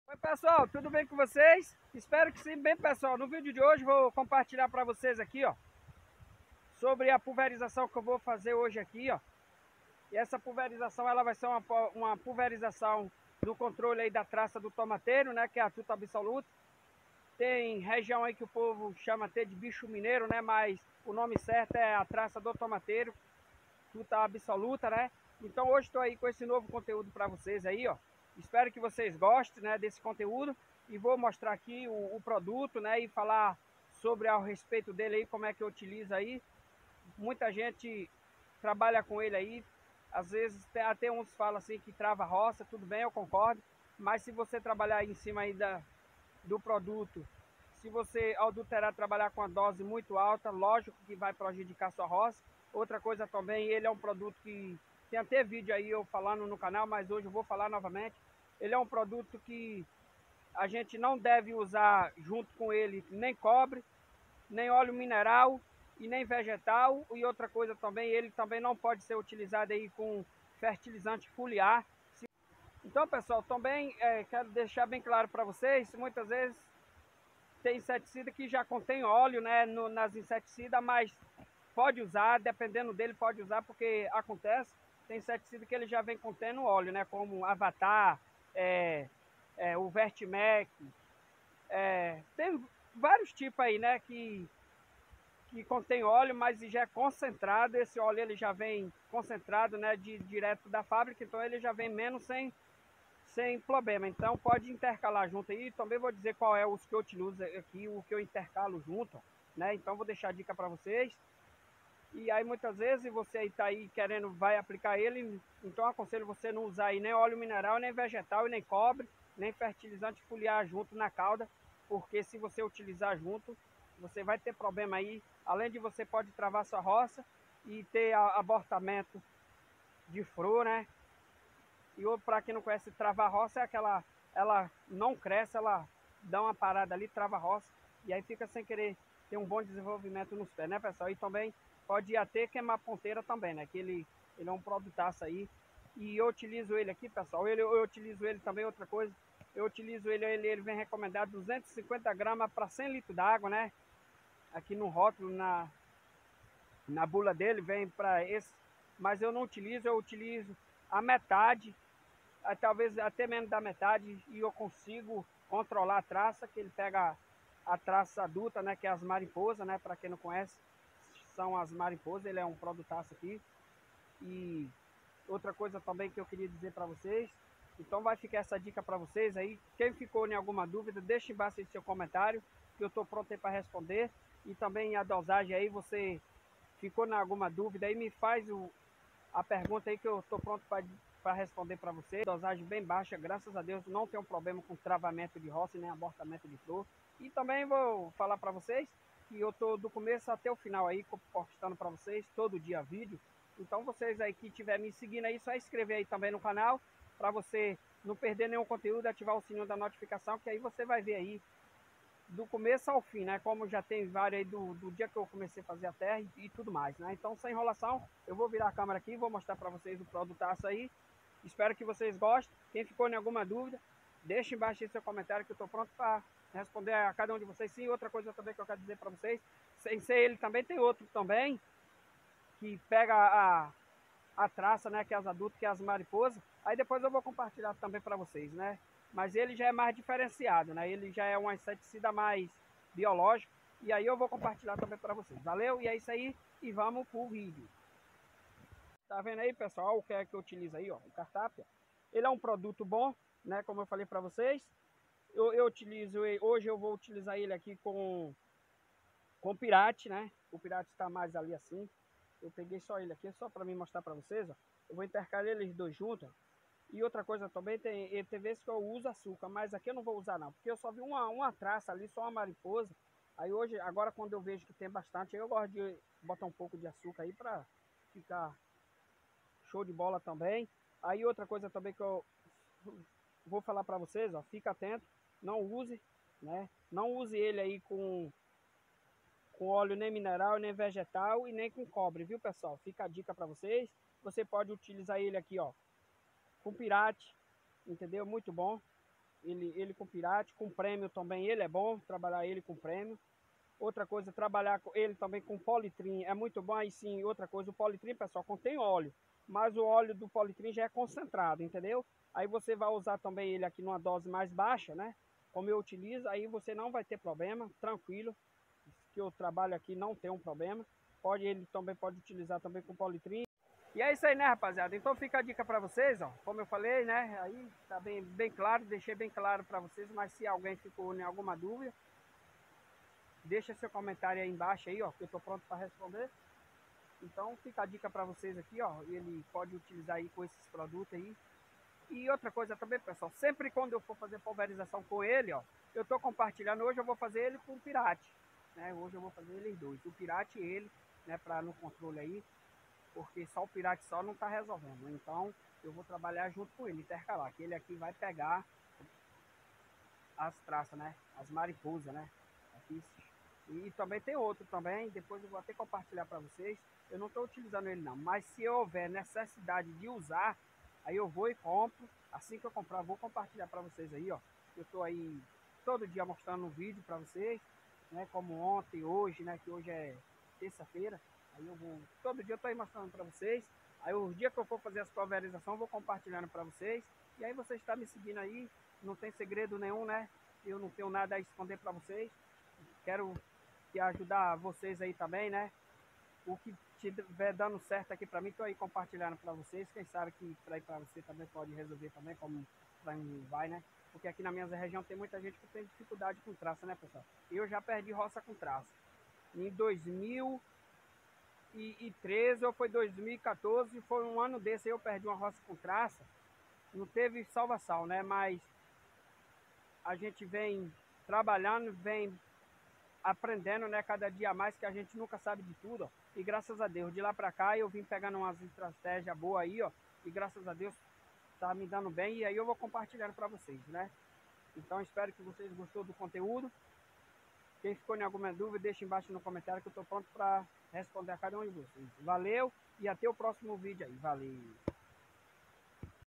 como controlar traça do tomateiro (tuta absoluta)no tomate
fazendo pulverização no controle da traça do tomateiro(tuta absoluta)